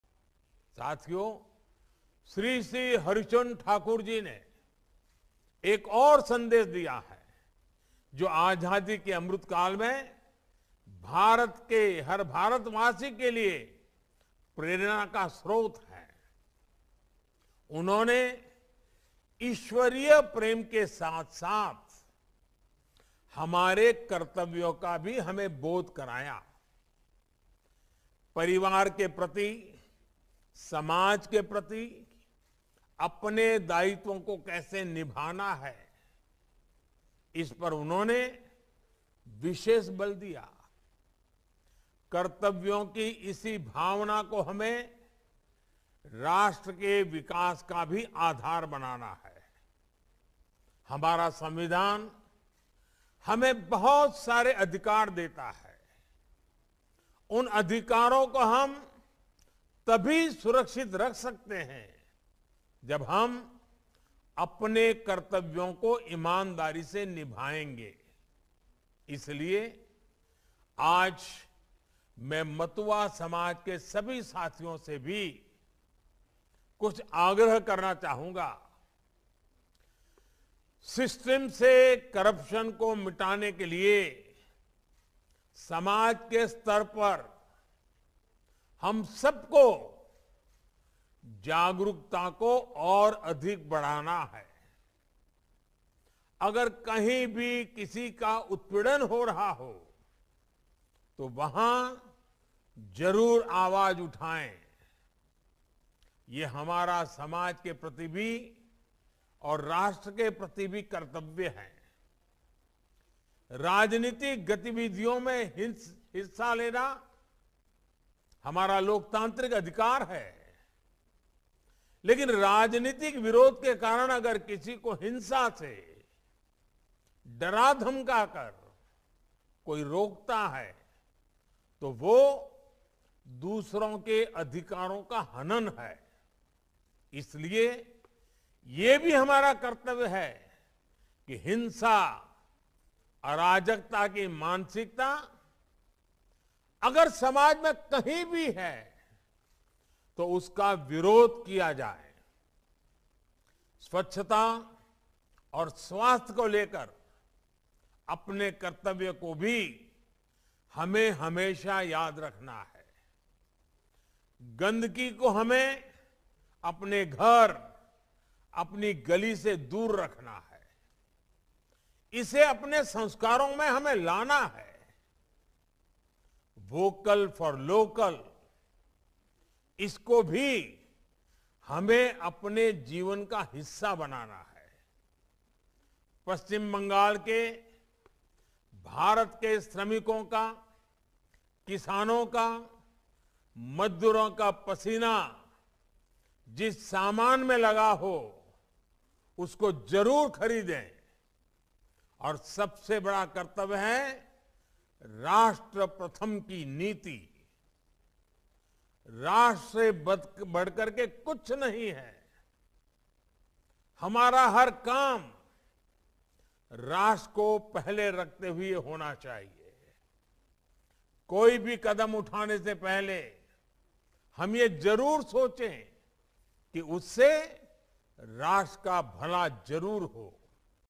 Nothing is more important than the nation: PM Modi
Prime Minister @NarendraModi addresses Matua Dharma Maha Mela at Shreedham Thakurnagar in West Bengal